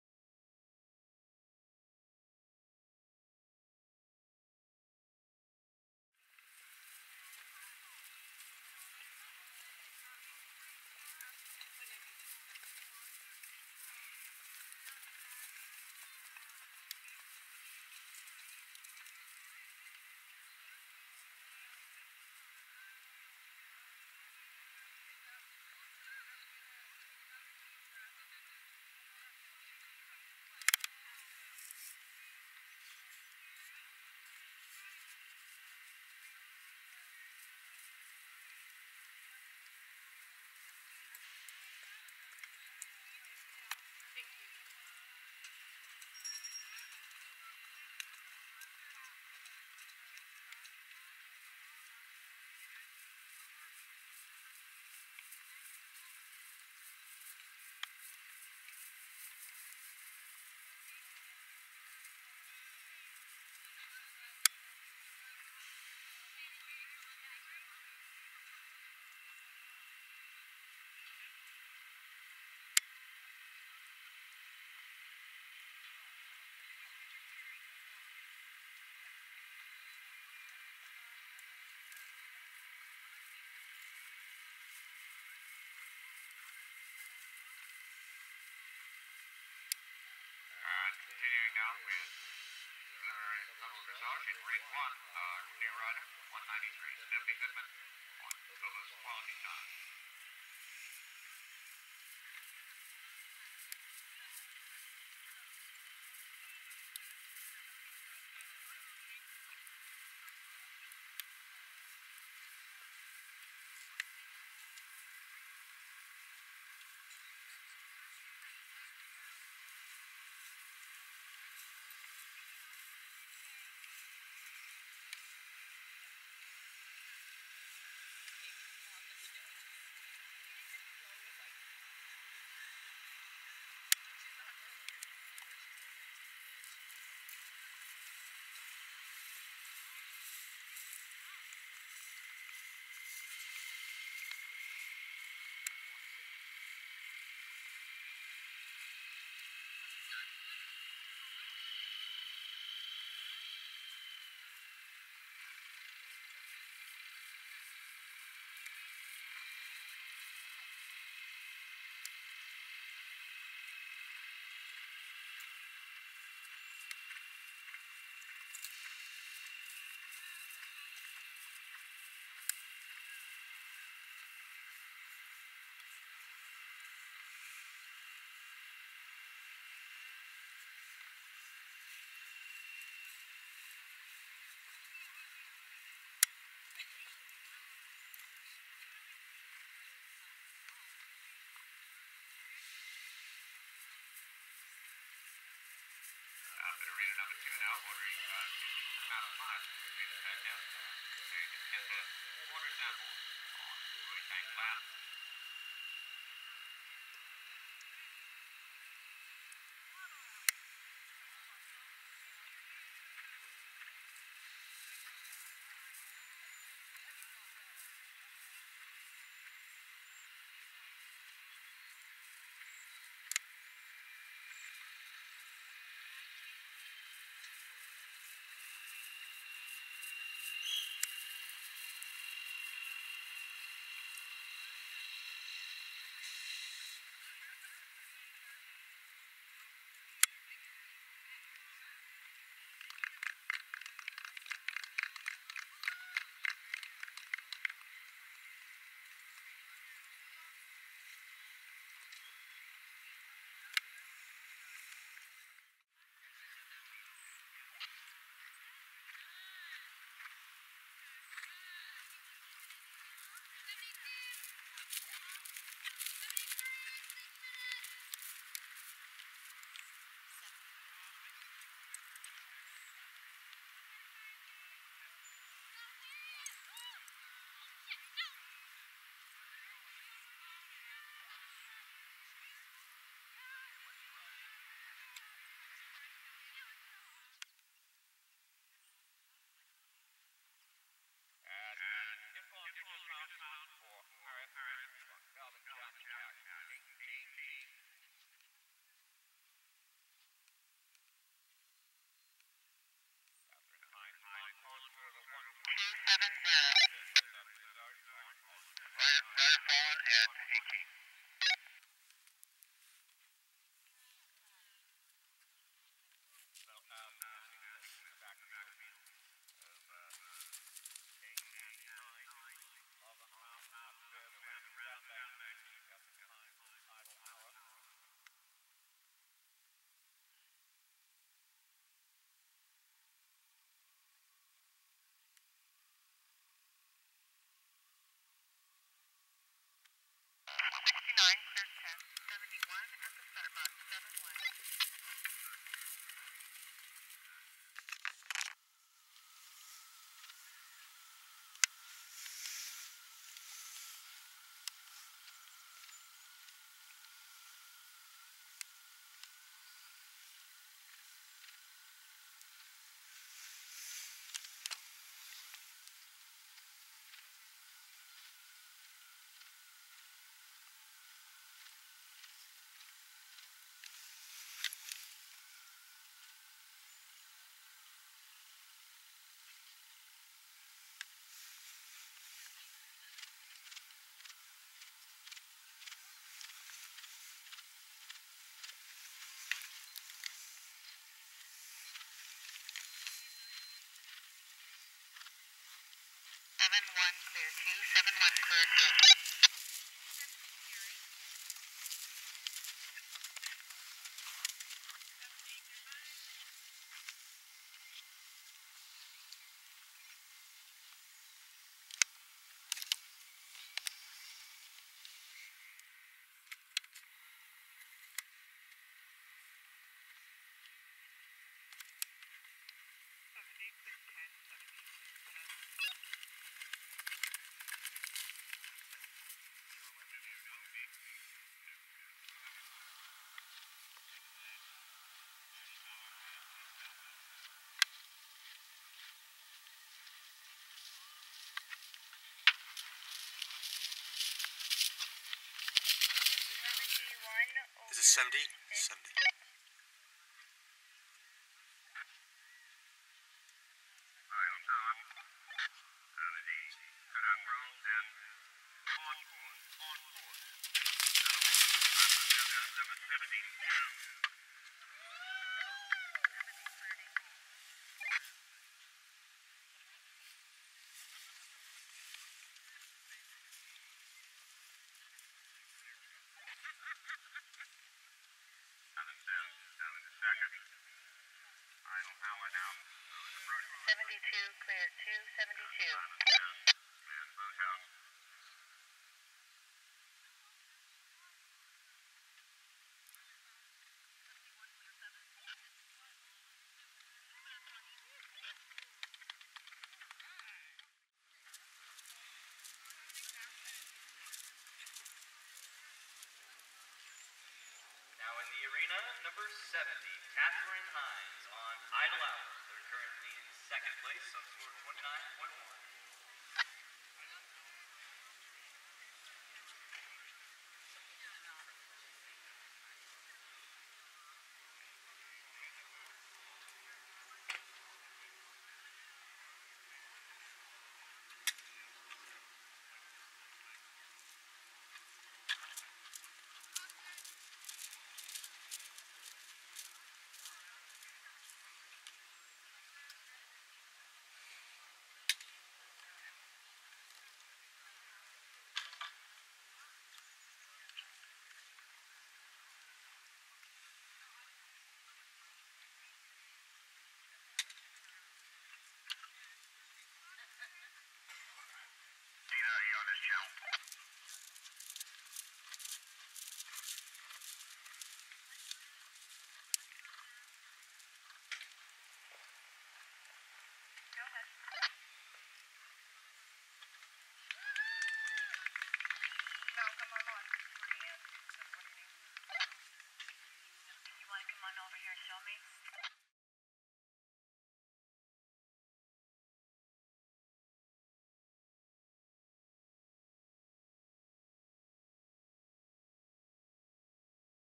Katherine Hinds; CFHT 2018
Katherine Hinds riding Chance in the Novice division
Caber Farm Horse Trials in Onalaska, WA, August 2018.